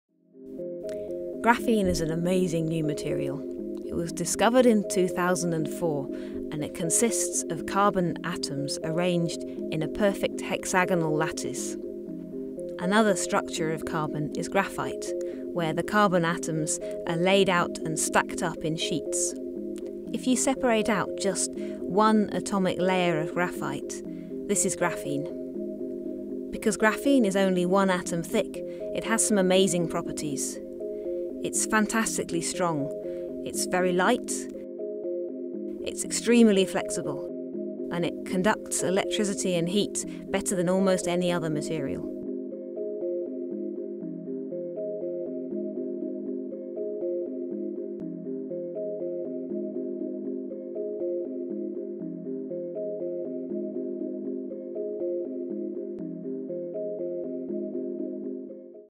What is graphene?
What is graphene? Graphene is the world's first 2D material which was initially isolated in 2004. It was discovered after scientists at The University of Manchester separated one atomic layer of graphite using simple sticky tape.

This short animation takes a look at how graphene was isolated and answers the widely asked question - What is graphene?

Narrated by Professor Sarah Haigh, Professor of Materials Characterisation. Her research focuses on the structure and properties of nanomaterials using high resolution transmission electron microscope (TEM) imaging and spectroscopic analysis.